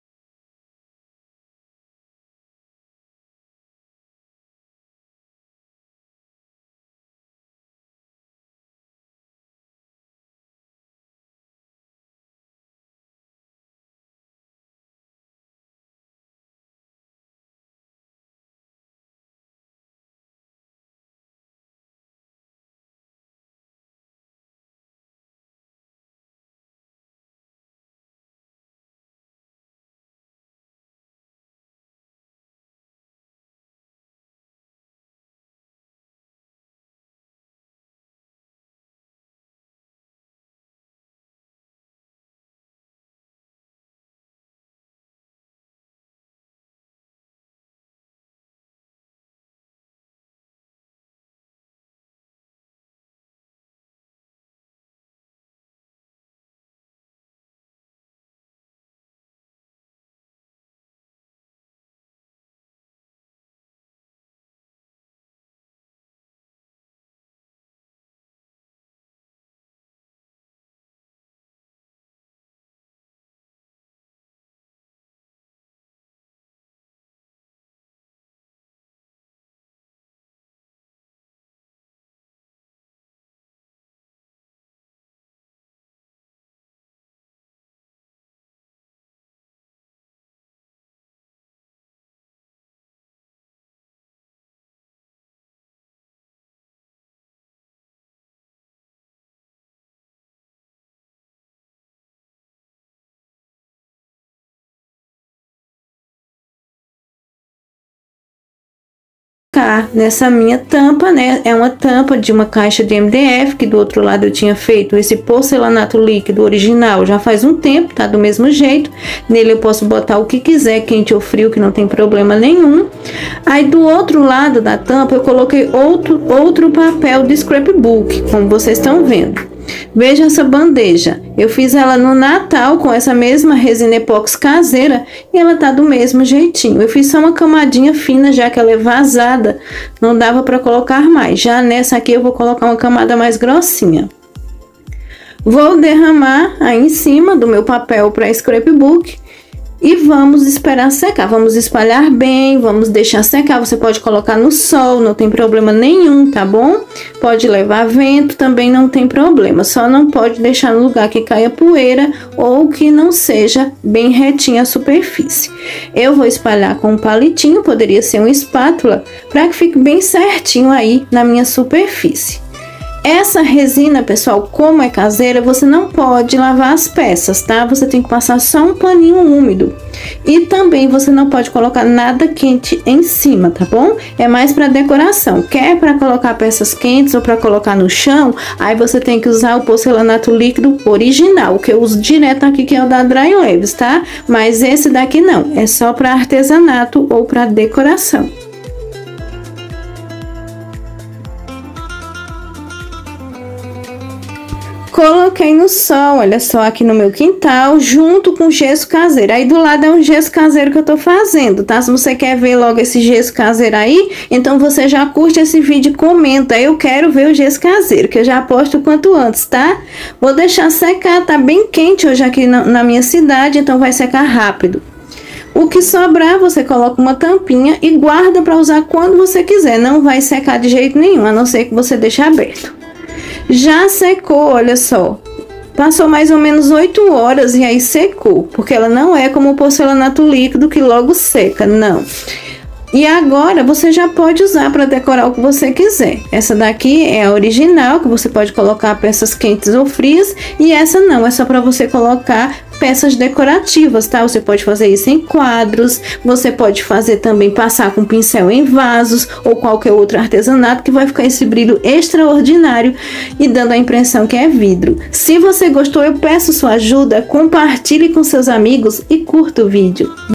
VIDRO LÍQUIDO CASEIRO muito fácil de fazer
Nesse vídeo mostro como é fácil de fazer vidro líquido caseiro para ser usado em artesanato
Materiais
Cola de silicone
Álcool líquido